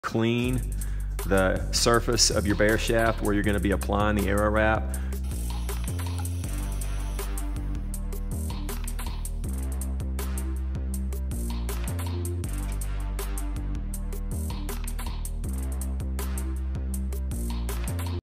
Clean the arrow shaft surface FIRST! [arrow wrap tips] | N1 Outdoors
Don't forget to clean your arrow shafts before applying arrow wraps! You need to do this to ensure proper adhesion!

**OTHER HELPFUL LINKS FOR HUNTERS BELOW**